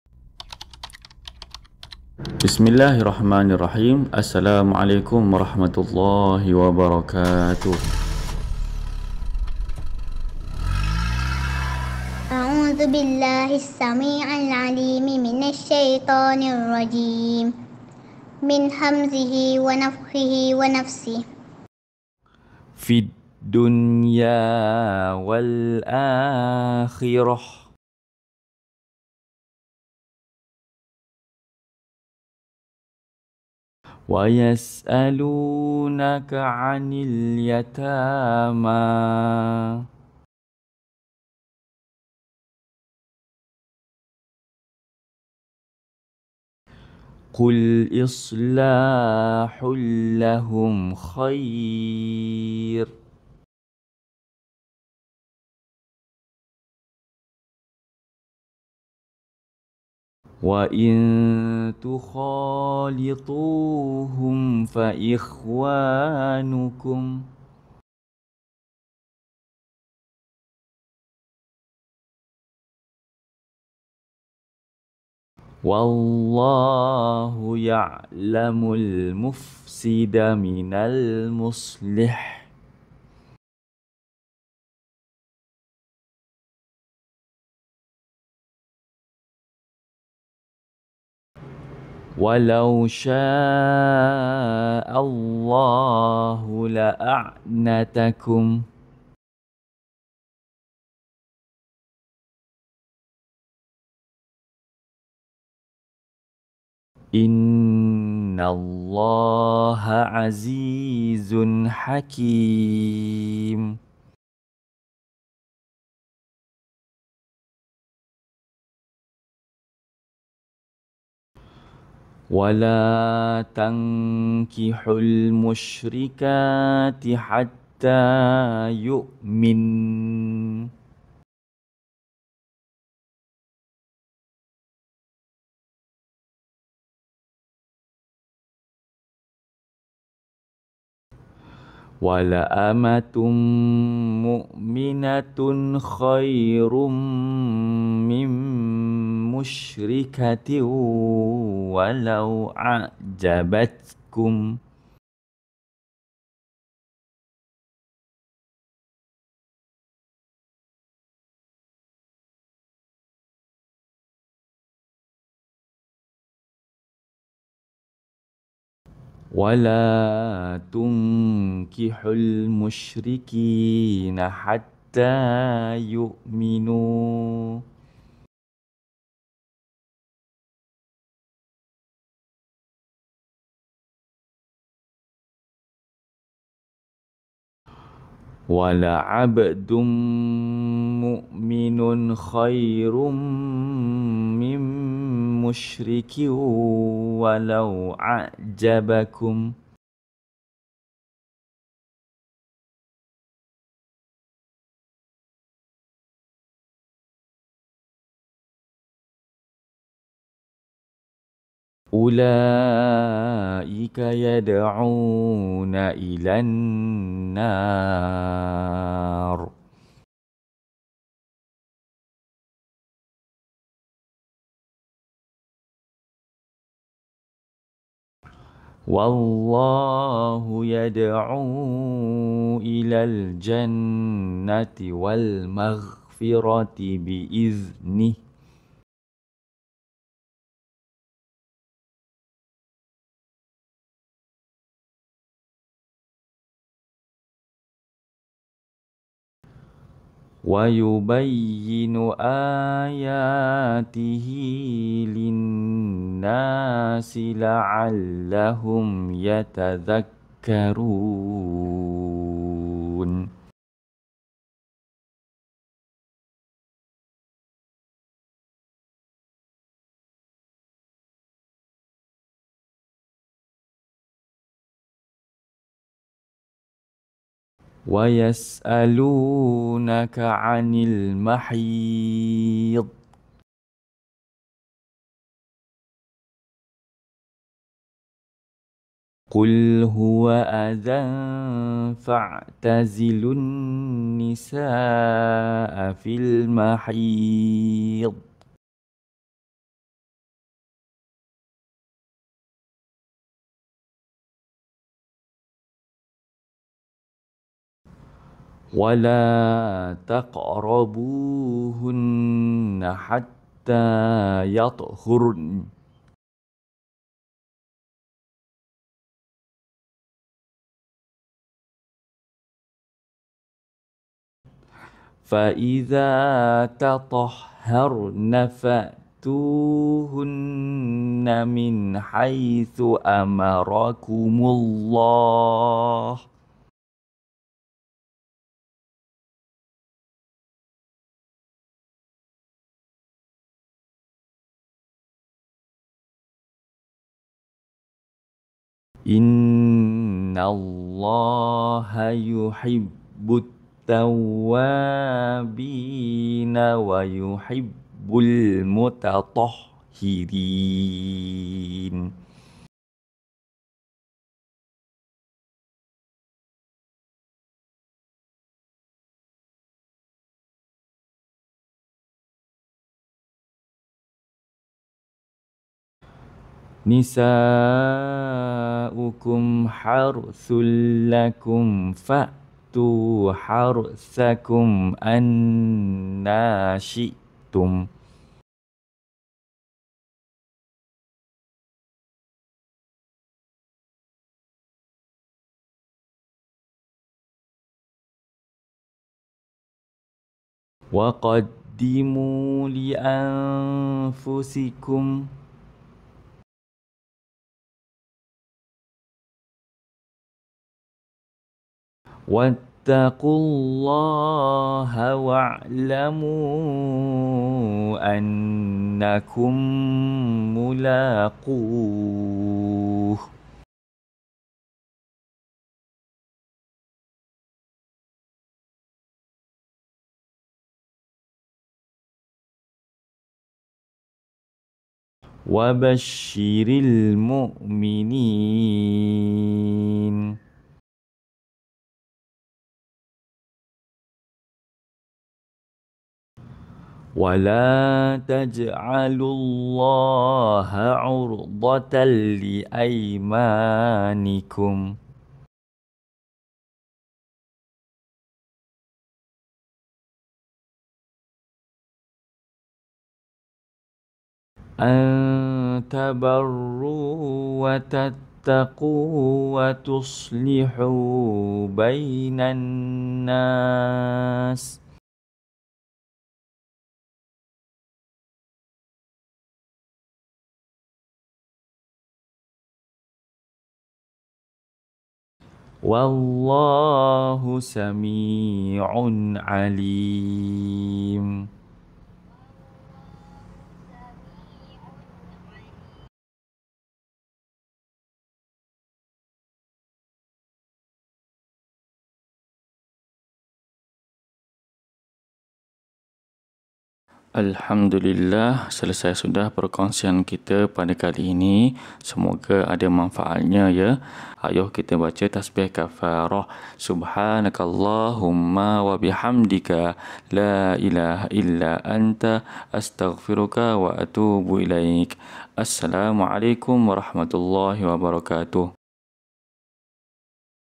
(Page 35) BELAJAR NGAJI QURAN Tadarus | Surah Al-Baqarah | Ayat 220 - 224 | LENGKAP TERJEMAHAN
Siri Tadarus yang dijalankan secara Online Google Meet bersama Kelas Tartil Al-Quran

🧑🏼‍🏫 Kaedah Tadarus :
Guru baca dahulu = Kemudian pelajar ikut bacaan guru.

📖 Al-Quran :
Digalakkan pelajar membaca menggunakan Al-Quran yang ada terjemahan ayat.